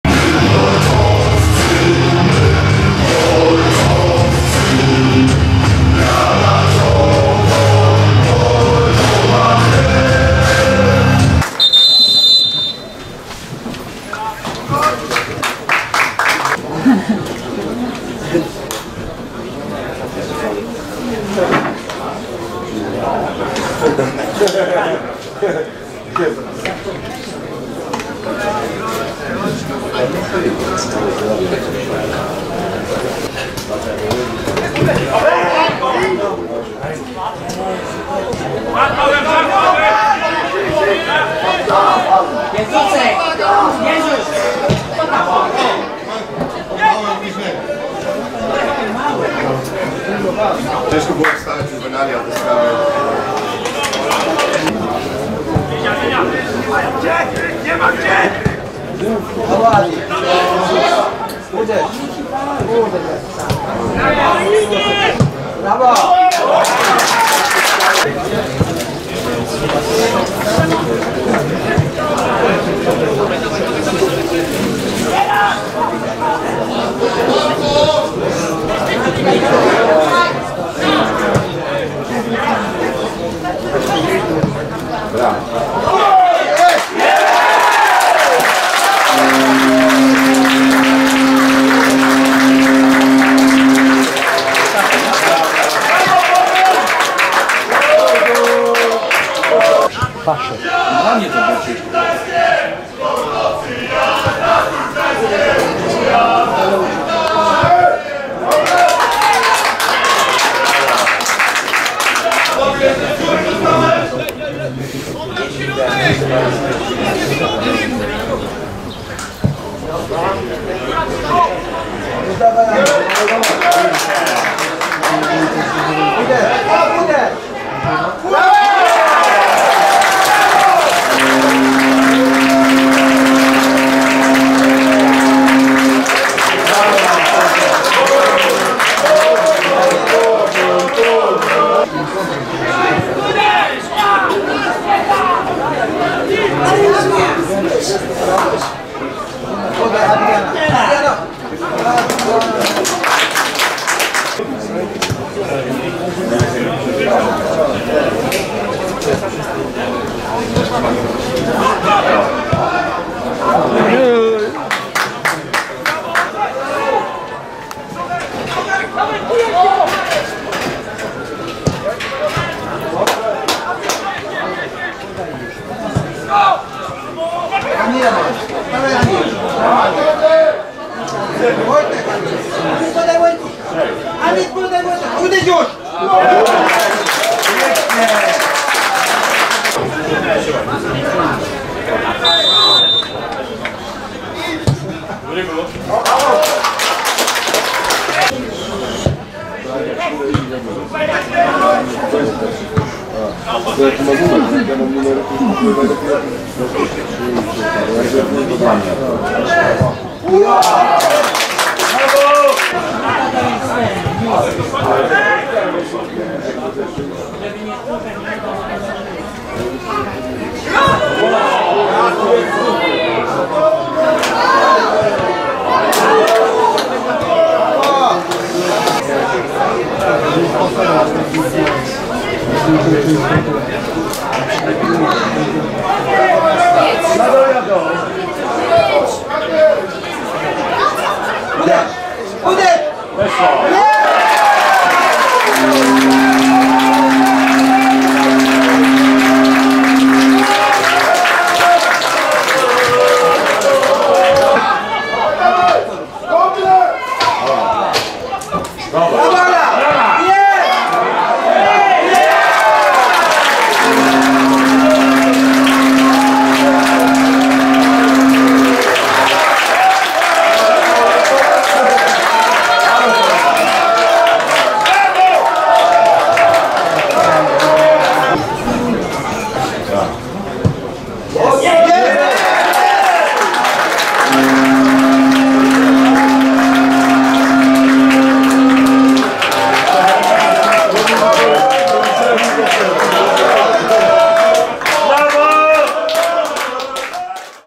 Skrót: Pogoń II Szczecin - Gedania Gdańsk 5:0
To był koncert w wykonaniu zawodników drugiego zespołu Pogoni! W meczu na szczycie III ligi granatowo-bordowi pokonali Gedanię Gdańsk 5:0 i wskoczyli na pozycję wicelidera III ligi.

29. kolejka III ligi
Pogoń II Szczecin - Gedania Gdańsk 5:0 (2:0)
1:0 Amin Doustali 16'
2:0 Kacper Łukasiak 32'
3:0 Olaf Korczakowski 56'
4:0 Wojciech Lisowski 58'
5:0 Amin Doustali 78'

Pogoń II Szczecin: Klebianiuk - Boniecki (44' Kwiecień), Lisowski, Białczyk, Pach (65' Góral) - Kalenik, Smoliński (65' Starzycki) - Przyborek (71' Mierzwa), Łukasiak, Korczakowski (71' Bąk) - Doustali